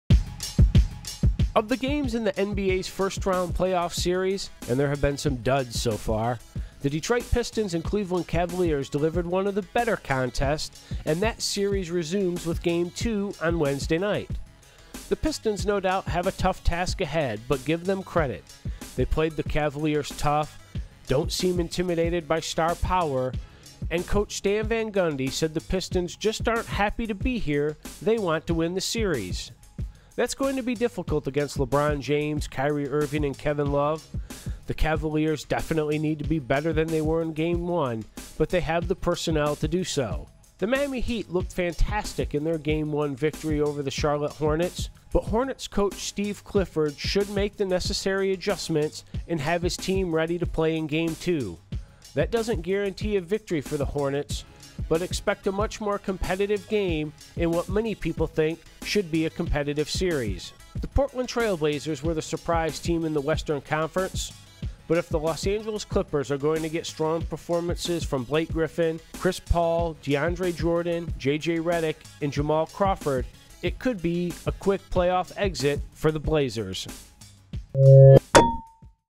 What to watch in Wednesday playoff action
USA TODAY Sports' Jeff Zillgitt breaks down the top story lines in Wednesday nights NBA playoff action.